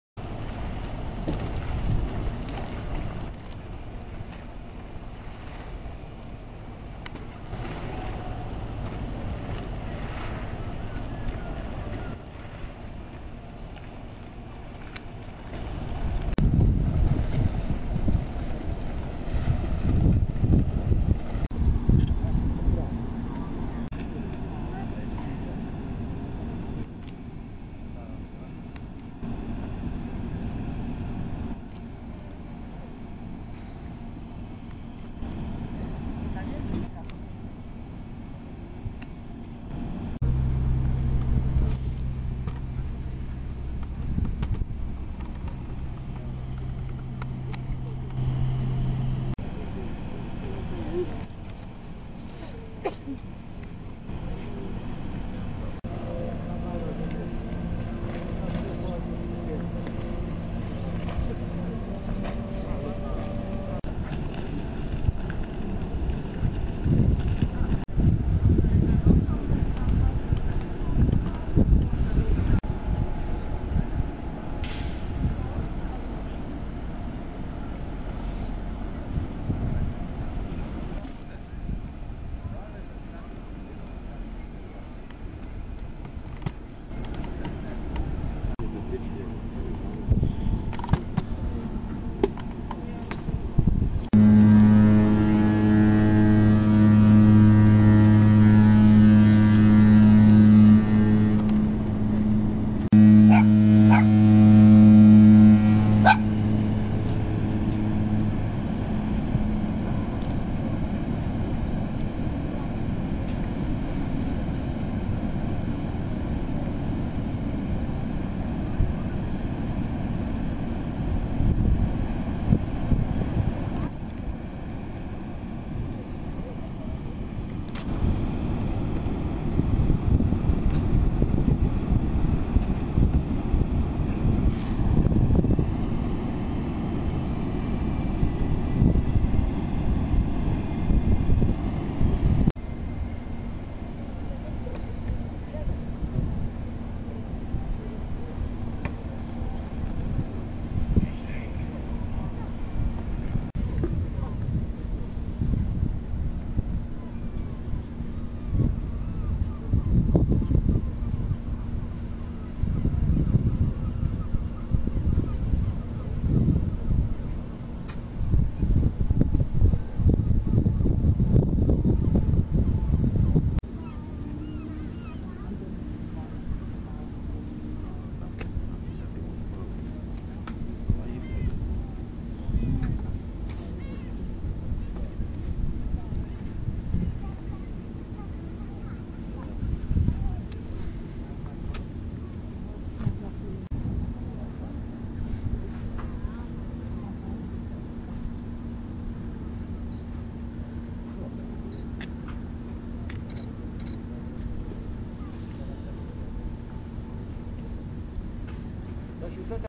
Ogromny prom rejsowy zawitał w porcie Gdynia. Wycieczkowiec Mein Schiff 4 w Gdyni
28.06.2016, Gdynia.
Wielki wycieczkowiec zawitał w gdyńskim porcie. We wtorek po godz. 19 wypłynął w dalszą podróż- następny przystanek na Litwie.